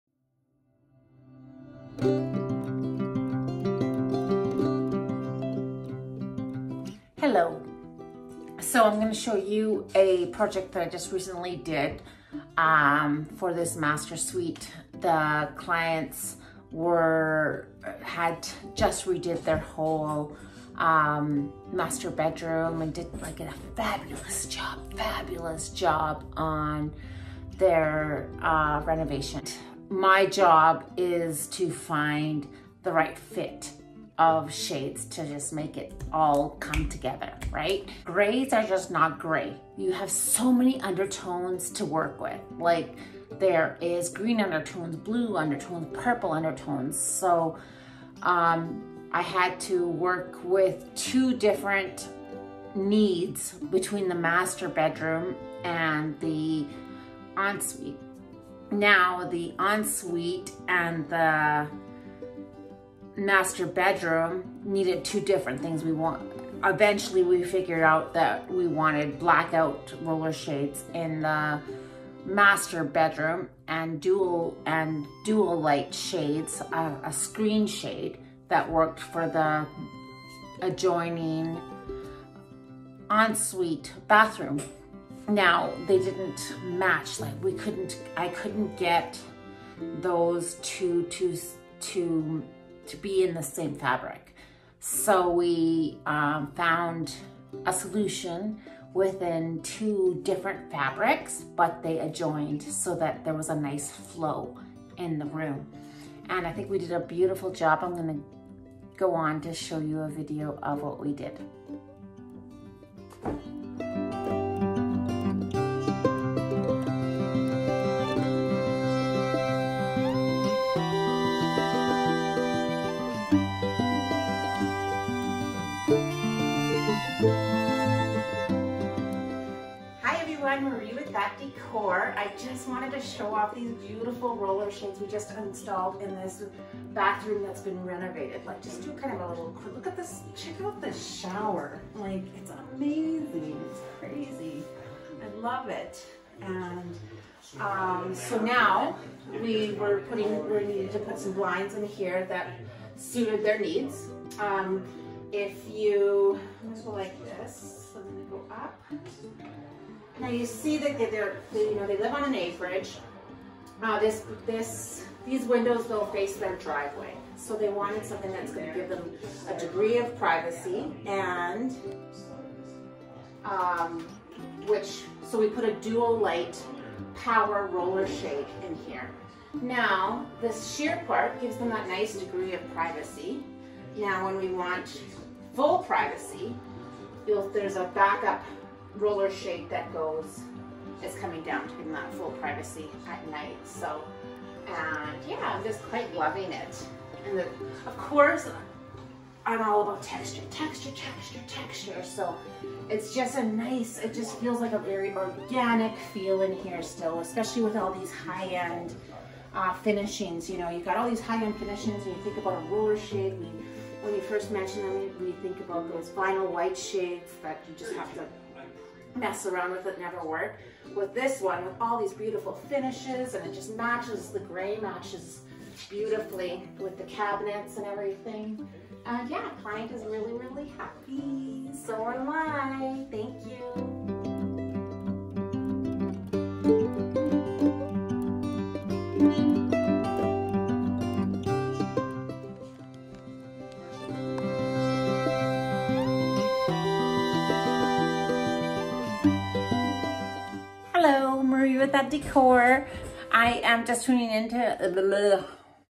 DuoLite Automated Roller Shades by Hunter Douglas
This is my attempt to tell a story about how window coverings make a difference in your room.  Since they are built to suit your own unique needs. There is so many factors to look at within the capabilities of what a shade can do.  In this project we were successful in bringing two different unique needs between two rooms.